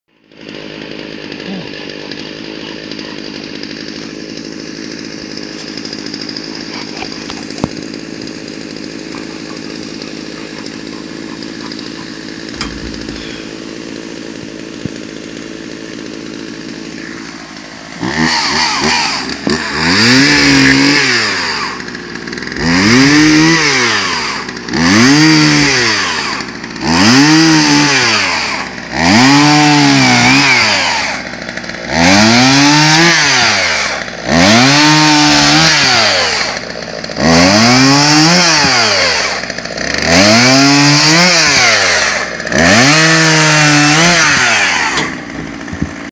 The EZ Sawbuck Demonstration
Cutting up firewood doesn't have to be an all day chore. With the EZ Sawbuck, you can have enough firewood for the night in just a couple minutes!